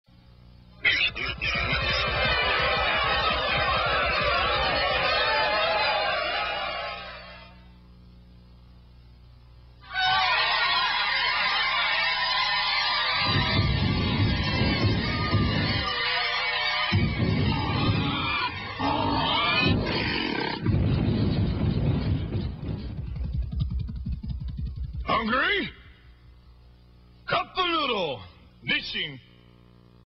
Nissin Cup-O-Noodle Giant Warthog
Another installment of the series of Kim Blanchette's Cup-O-Noodle Nissin stop motion commercials from 1992.